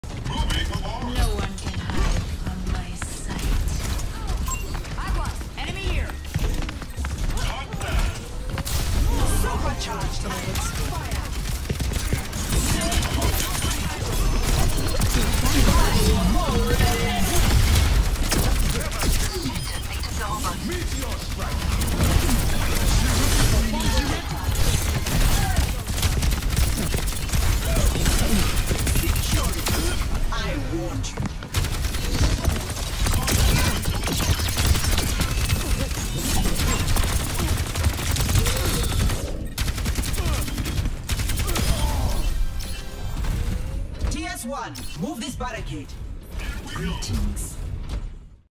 Resilience!
Maintaining the push of the robot when the rest of my team perished.